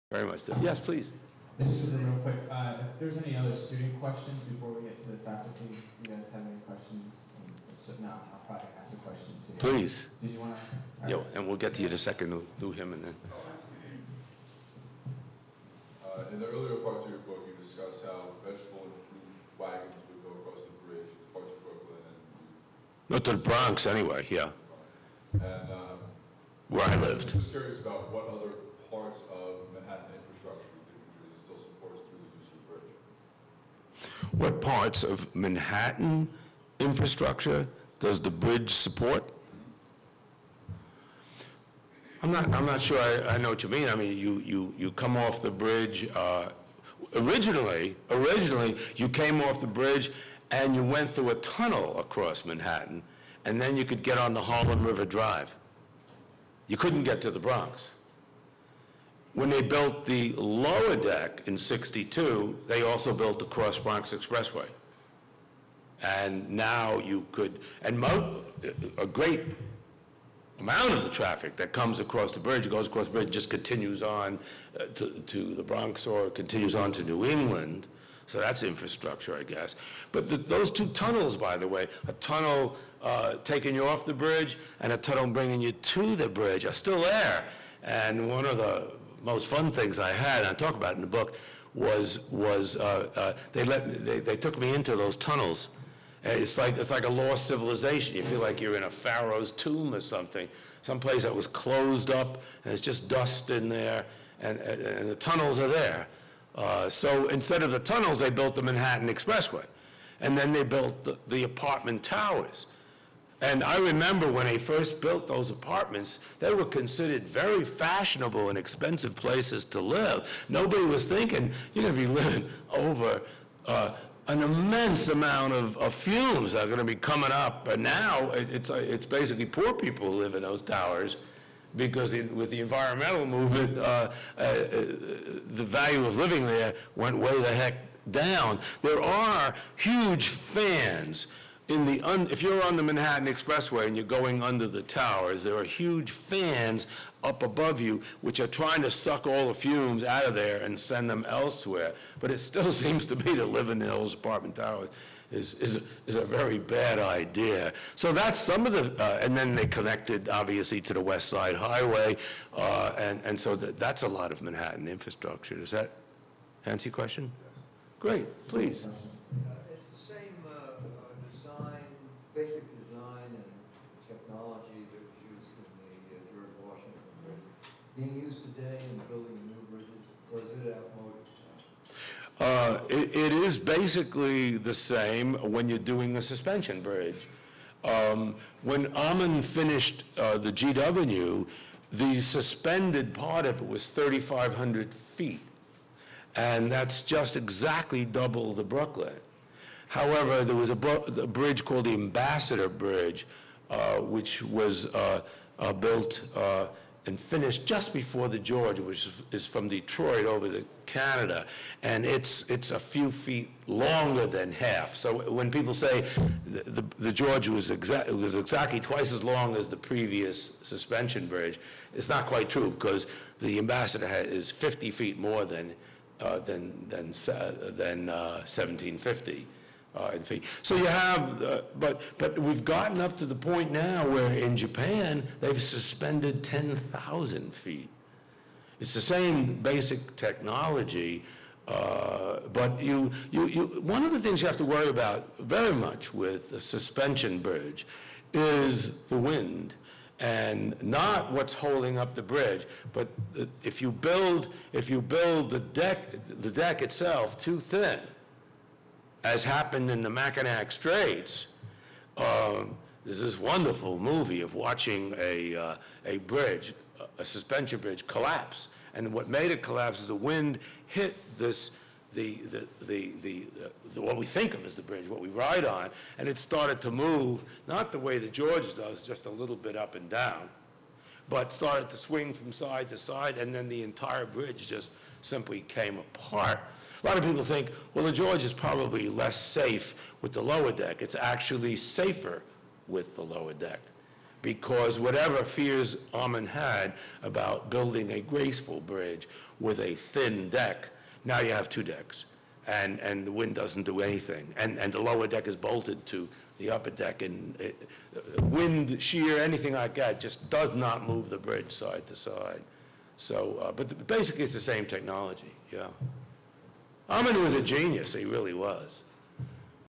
Part 9, Michael Rockland at FDU (2/6/09)
Q & A (cont.). How has the Bridge affected other infrastructure?; the tunnels leading away from the Bridge; the lengths of suspension bridges around the world; what happens when a suspension bridge's deck is too thin.